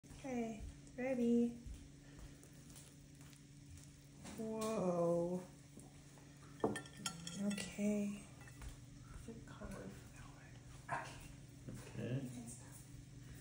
Opening a Custard Apple
Custard apples come in a couple varieties in Taiwan - the big custard apple (shown here) and the pineapple variety. For this one, we first bought it before it was ripe and the fruit was still quite hard. The merchant told us to wait 3 days before eating and, like clockwork, on day 3 it was so soft to the touch you could literally squeeze the entire thing to a pulp in your hand (don't recommend). Here it is being pulled open for consumption -- eat with a spoon by scooping the fleshy part out.